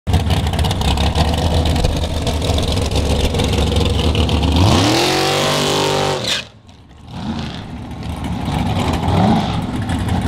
race 24 MAck Daddy
Love to see KiLeigh tuning on her own car while Mack DAddy is driving out the kinks